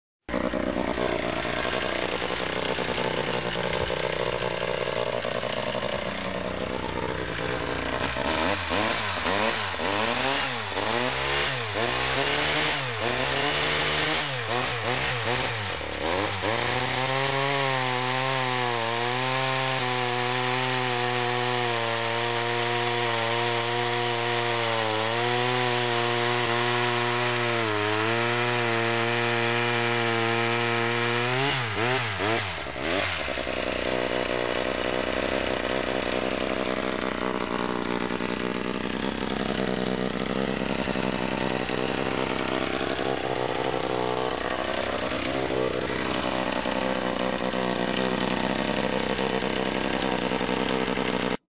Yanmar Rotary Engine Chainsaw 2
Short Test of the Yanmar Rotary Engine Chainsaw RH600A